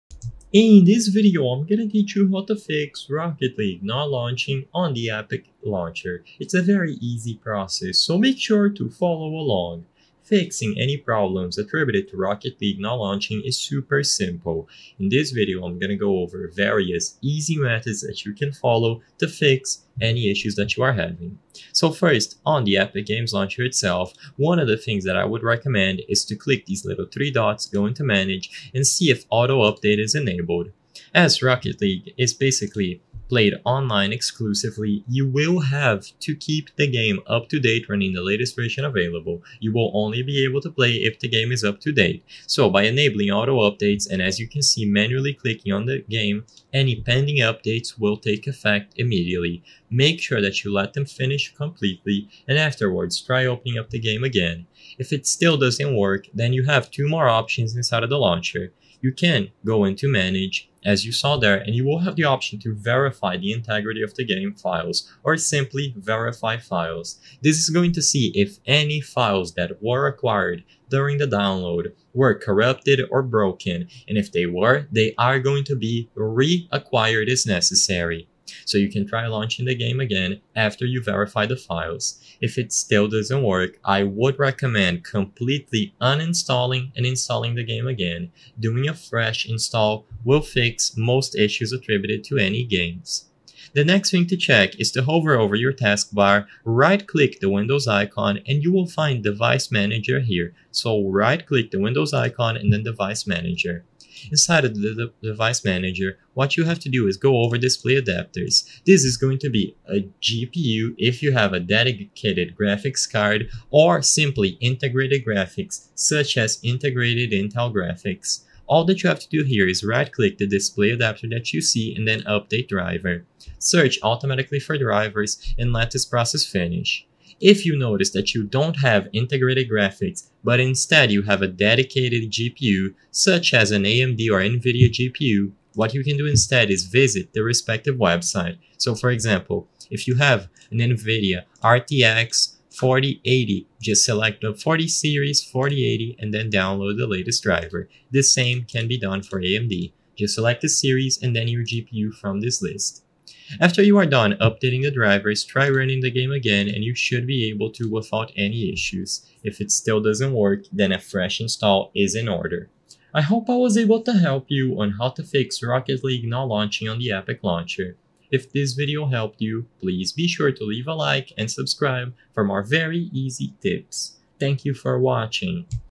How To FIX Rocket League Not Launching on Epic Launcher 2023/2025
In this video I will clear your doubts about how to fix rocket league not launching on epic launcher, and whether or not it is possible to do this.